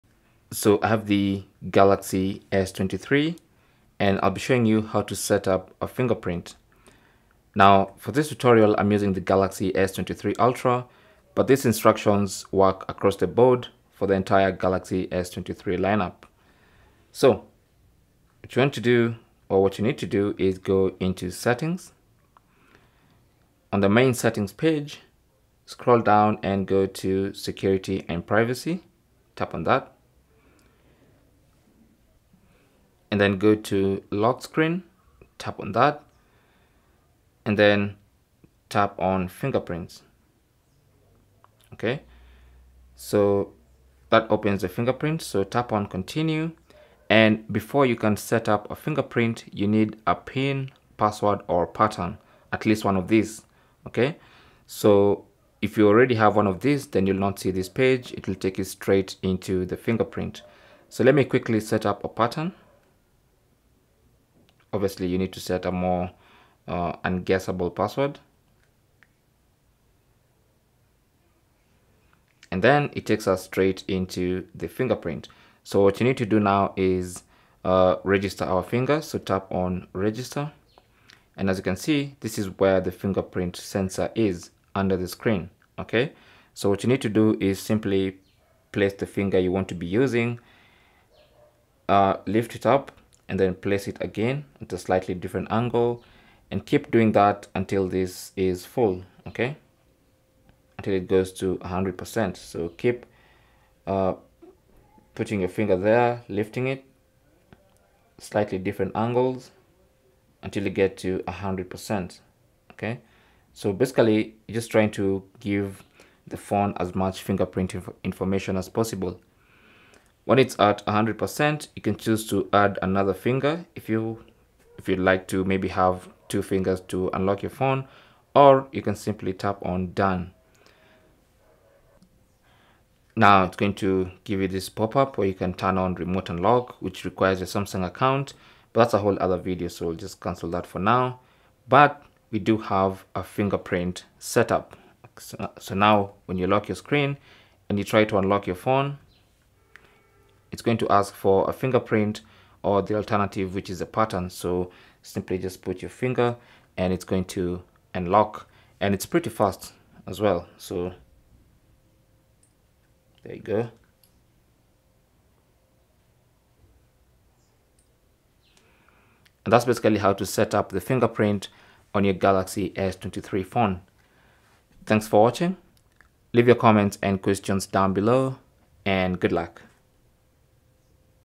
How To Set Up Fingerprint on Samsung Galaxy S23, S23+ and S23 Ultra
How To set up fingerprint on the Samsung Galaxy S23 Ultra. These instructions also work across the S23 lineup on the Galaxy S23 and Galaxy S23+.

You can register more than one finger and even rename the fingerprints in the Fingerprint menu of your Galaxy S23.

The Galaxy S23 series has an optical fingerprint scanner beneath the display.  Here's how to register your finger.

How To Add Fingerprint To Samsung Galaxy S23

0:00 - Intro
0:18 - How to get to Fingerprint lock screen menu
1:00 - Set up PIN, Pattern or Password
1:12 - Register fingerprint
2:10 - Remote Unlock
2:26 - Fingerprint unlock tests
2:49 - Outro

Cell Phone Tripod Adapter